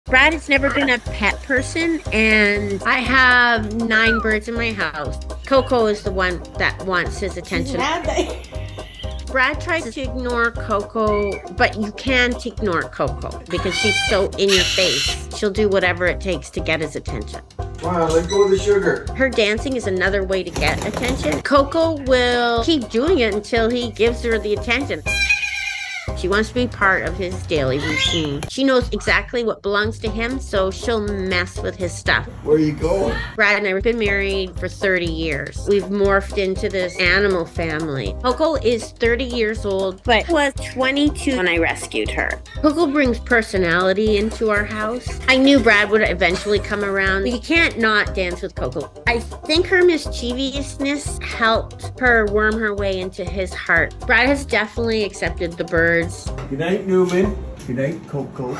30-Year-Old Bird Is Determined For Dad To Love Her | The Dodo
She won Dad over with the power of dance ✨ @danita4sure